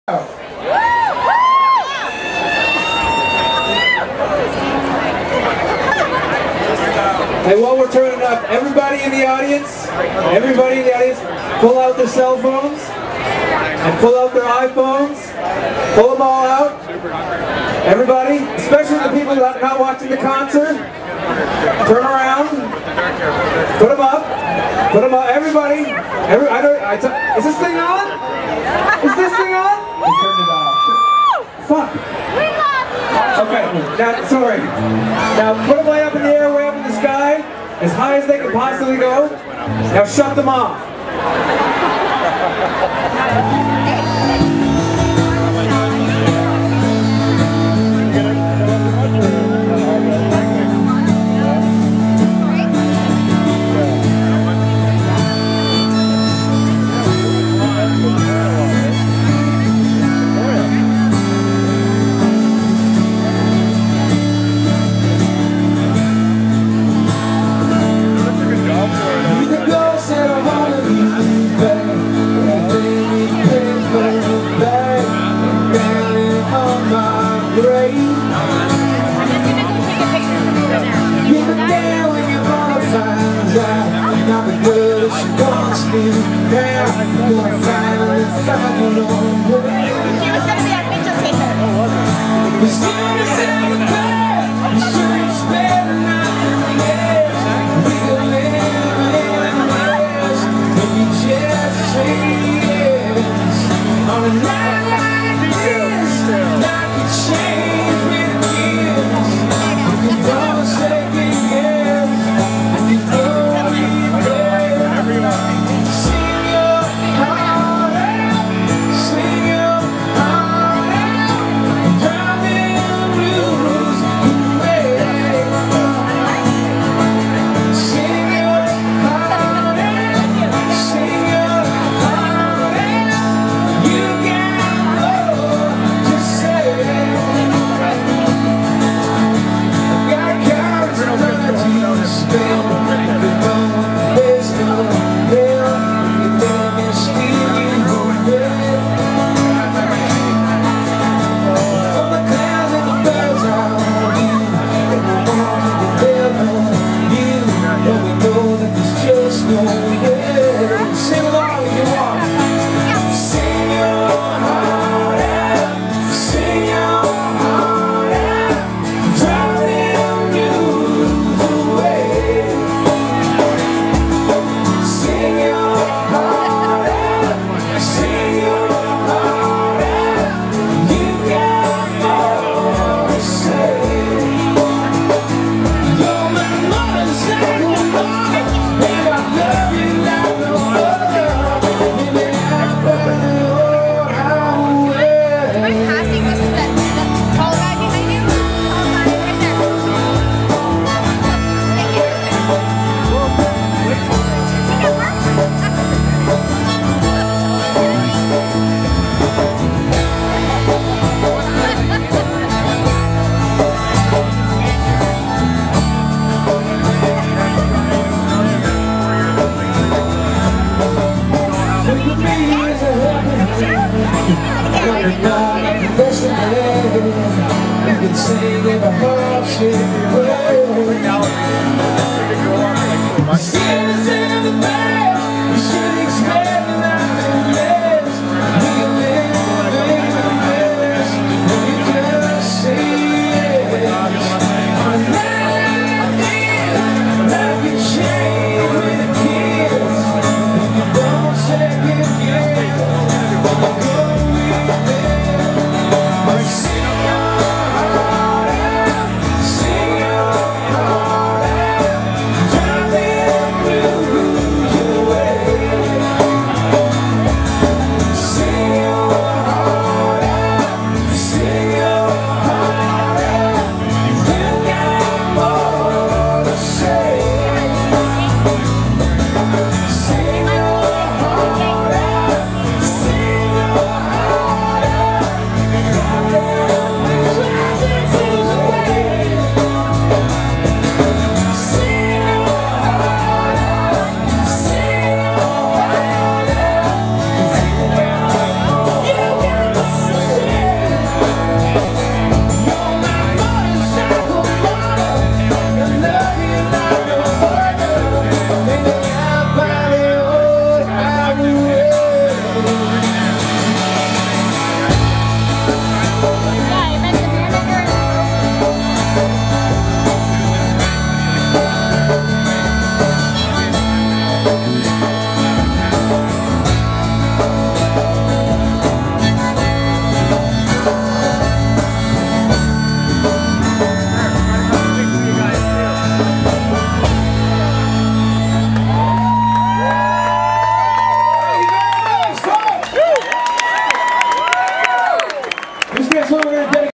The Trews Live at SHOREfest
The Trews play live at SHOREfest on English Bay on August 1st 2012